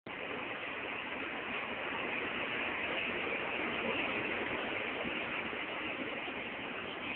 3.8.2009 - minibus driving on pavement - Minispot.org
Chai Wan MTR